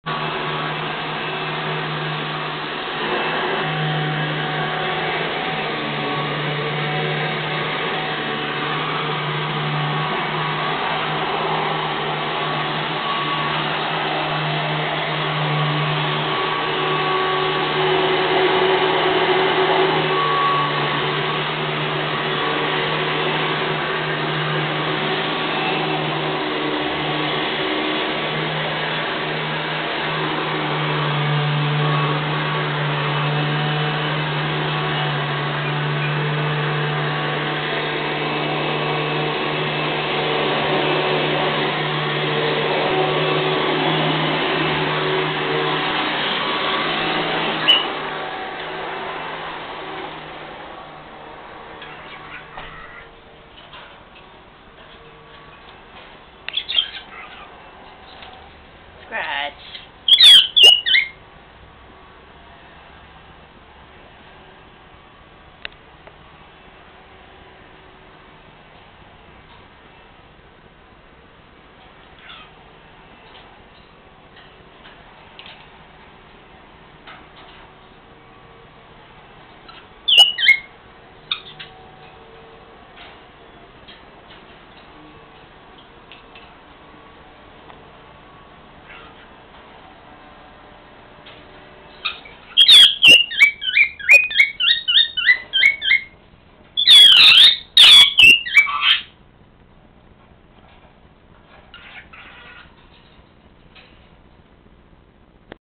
Scratchy taking a bath
This is what she does when you vaccuum.